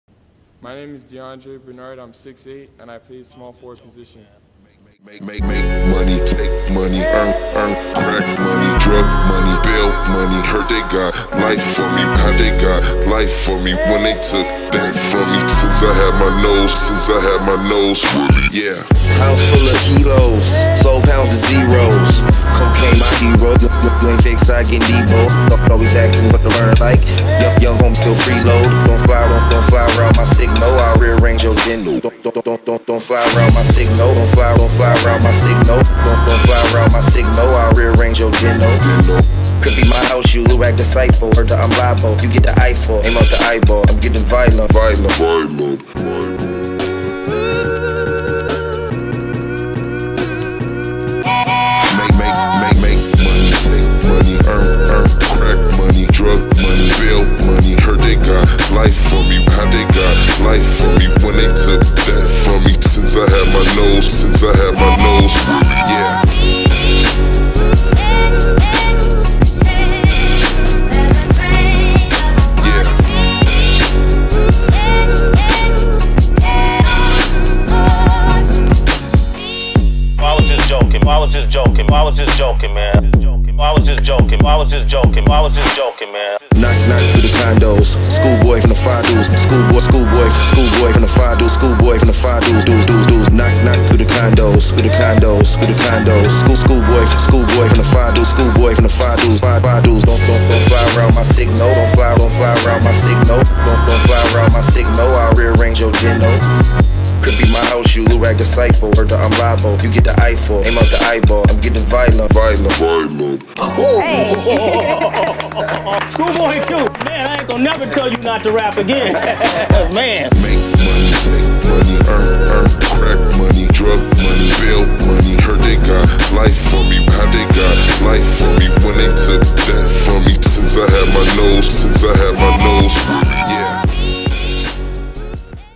BCSS: D'Andre Bernard Does Basketball
This video is about DAndre Bernard a 6'8" forward from Canada's Bill Crothers Prep Basketball Team.